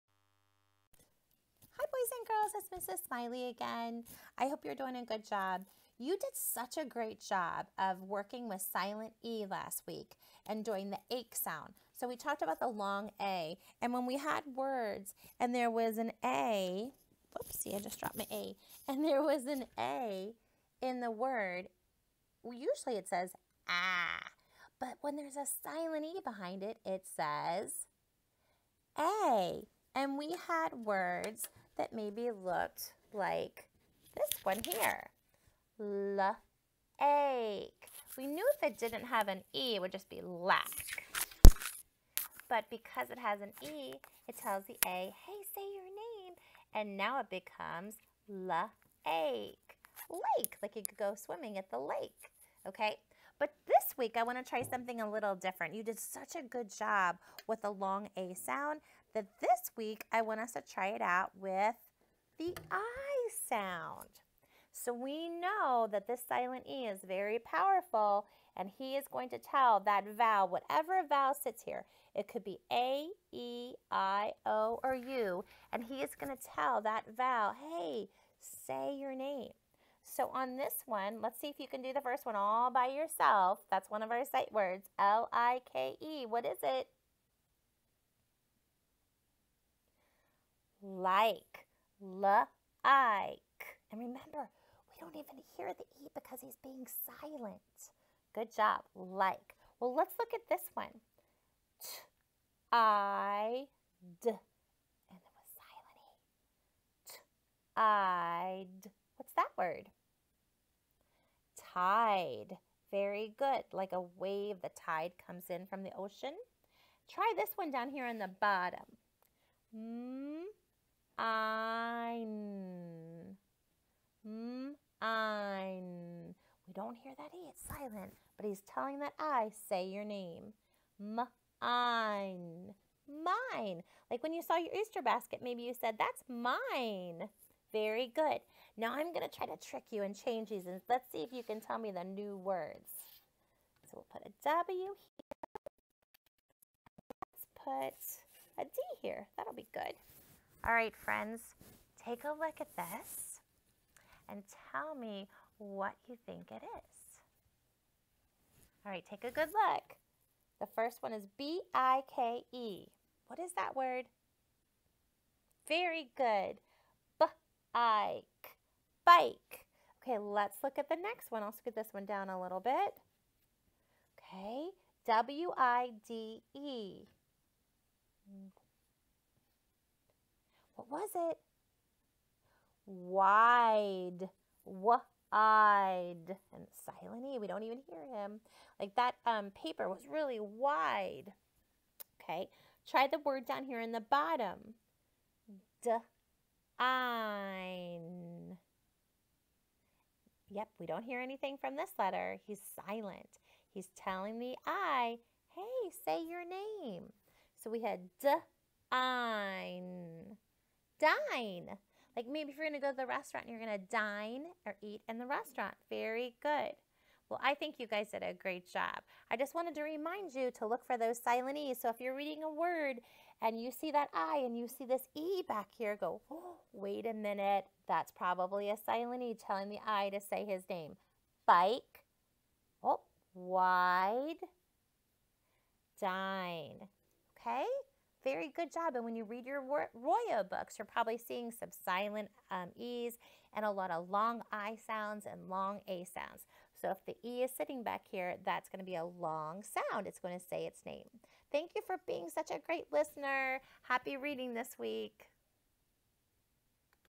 Long /i/ sound
Here is a lesson to teach the long /i/ sound.